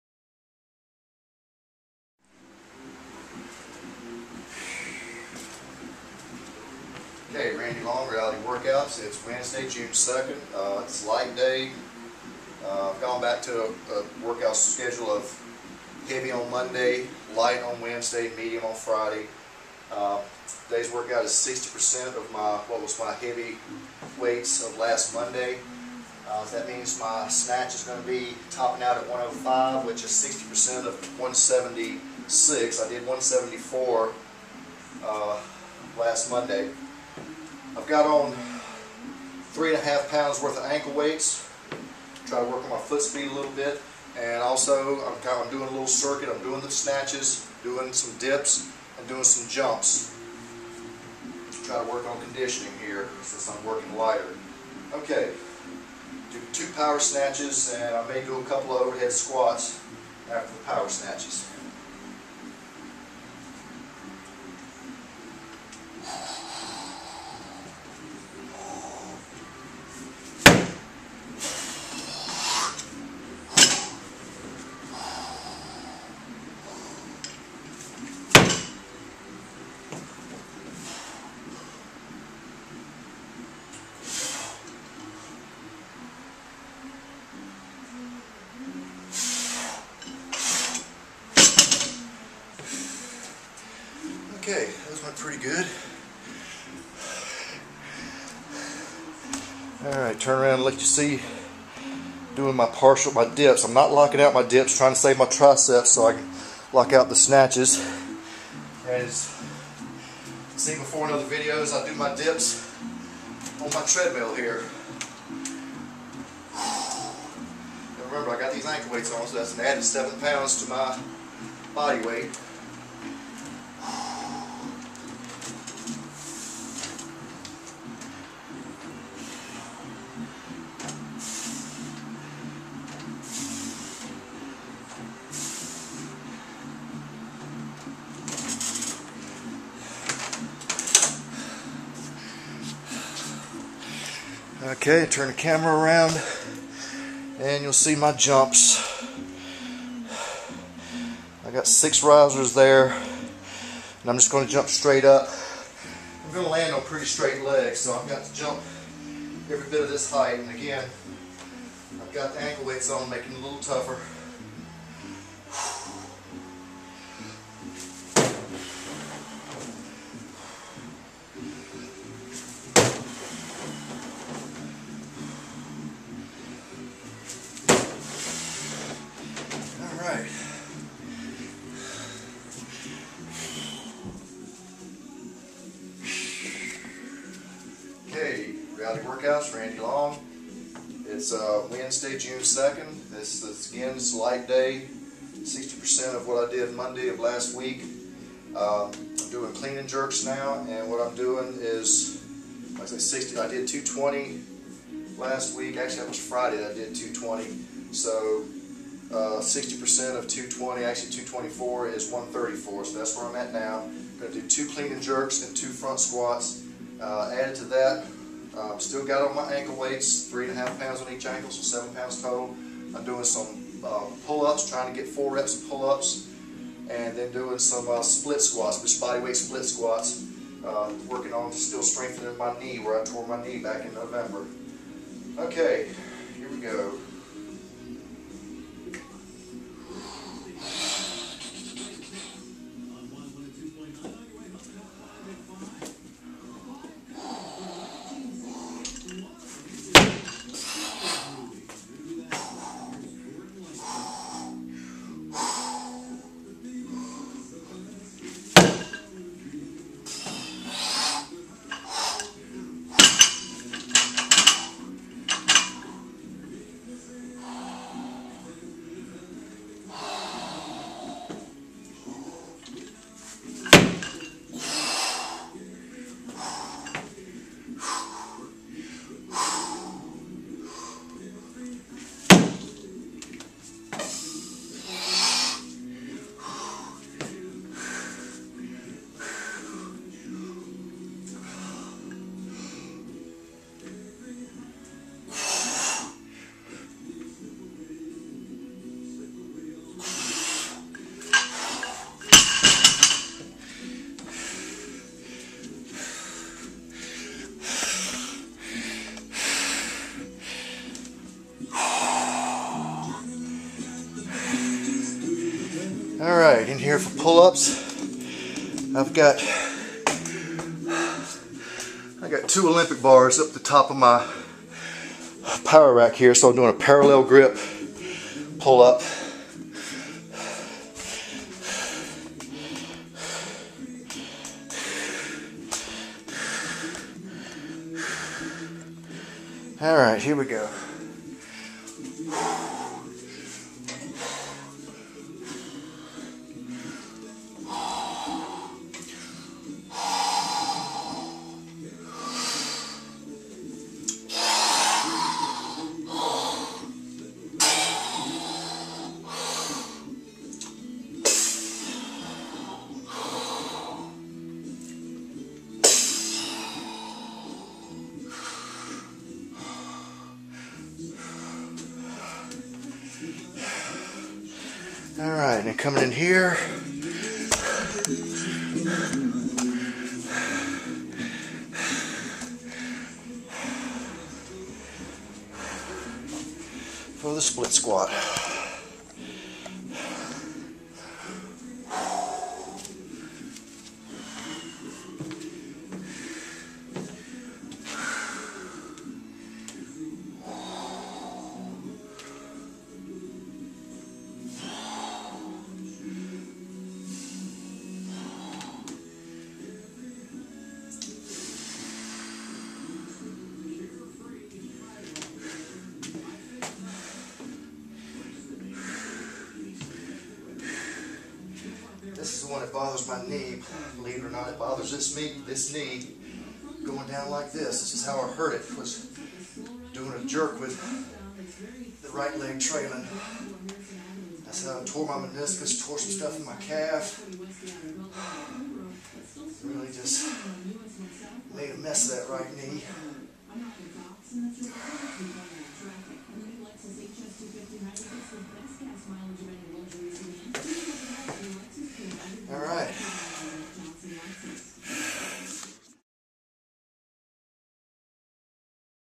Reality Workouts Randy Long 6/2/10 60% PS, PC+J Circuits
60% day, using 60% of my most recent max double wts. Wearing 3.5 lbs of ankle wts.
Working in a small circuit of Power Snatch/Ovehead Squat, Dips, and Jumps,  and Power Clean & Jerk, Pullups, and Split Squats.
3 rounds on each, plus warmups, so a lot of total reps on the dips, pullups, jumps and split squat.